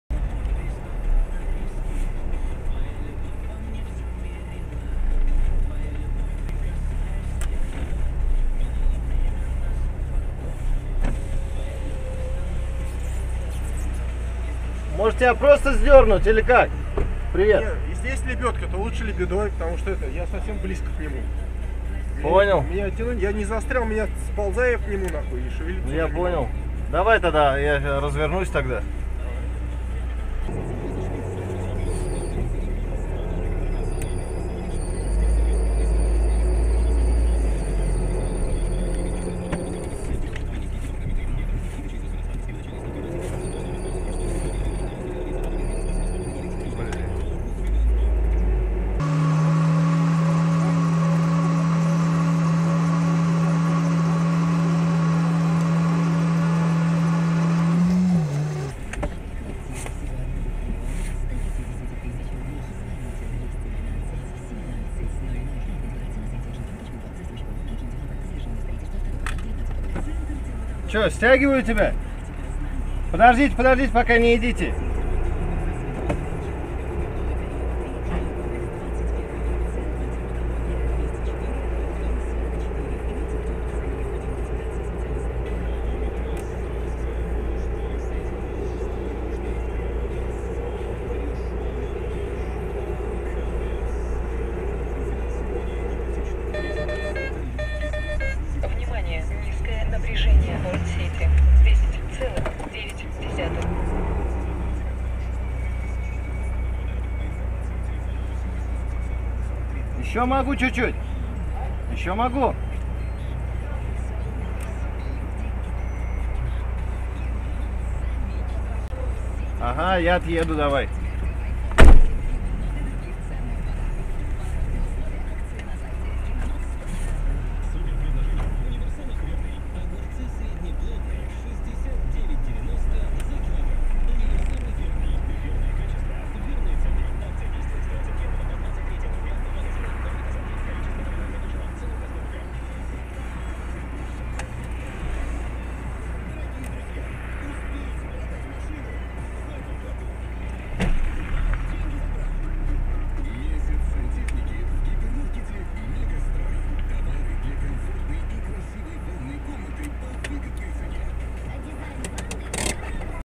Джип Алерт. помощь выезда с парковки
для разнообразия. бывает и вот так - парковка, полный привод, казалось бы чуть жижки и небольшой уклон, хорошо что человек опытный джипер и как его свозить начало на соседнюю машину, сразу перестал дергаться и позвонил